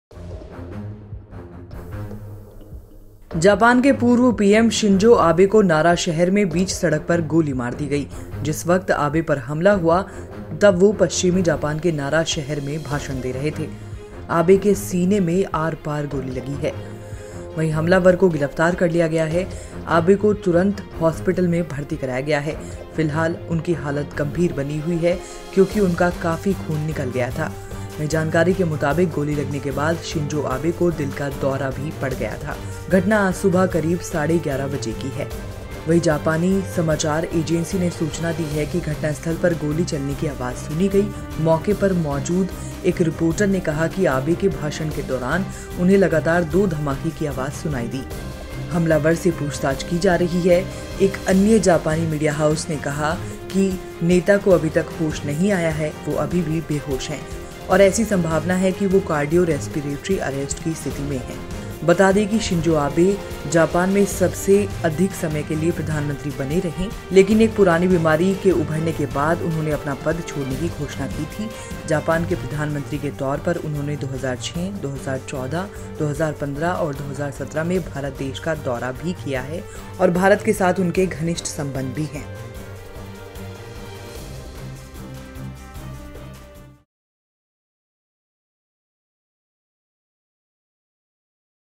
Shinzo Abe Shoot Updates: शिंजो आबे के सीने के आर-पार गोली, जापान के पूर्व पीएम पर हमले का Video
जापान के पूर्व पीएम शिंजो आबे को नारा शहर में बीच सड़क पर गोली मार दी गई। जिस वक्त आबे पर हमला हुआ पश्चिमी जापान के नारा शहर में वो भाषण दे रहे थे. आबे के सीने में गोली लगी है. हमलावर को गिरफ्तार कर लिया गया है. उनको तुरंत हॉस्पिटल में भर्ती कराया गया है.